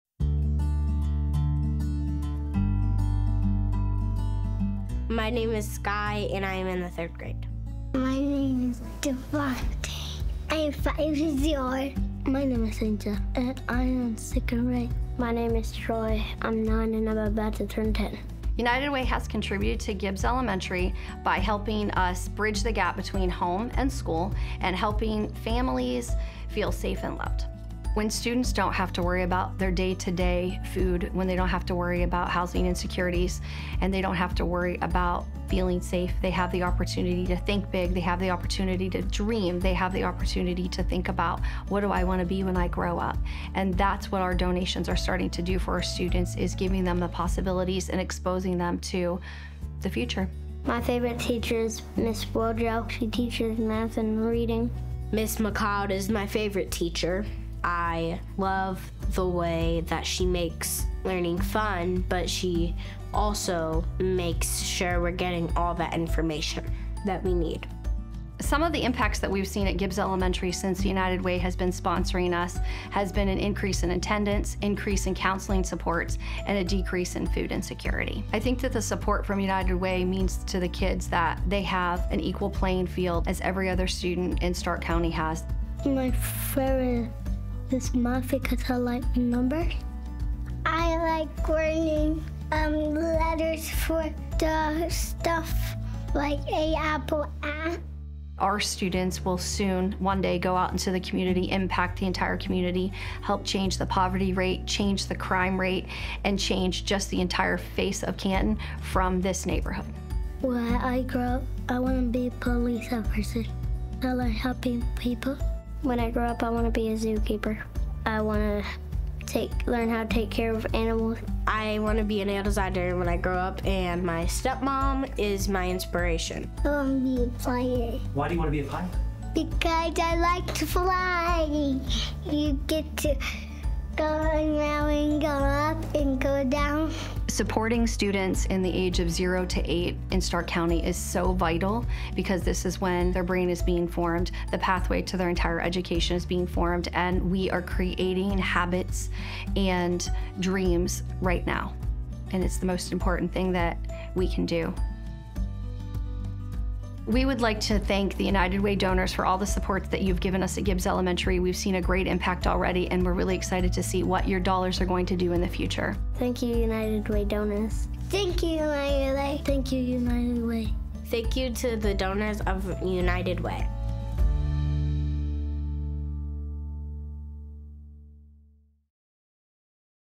When I Grow Up Starts Today
Watch as United Way of Greater Stark County partners with Gibbs Elementary School fueling the dreams and ambitions of young minds. Join us as we pave the path for children to reach their fullest potential and aspirations.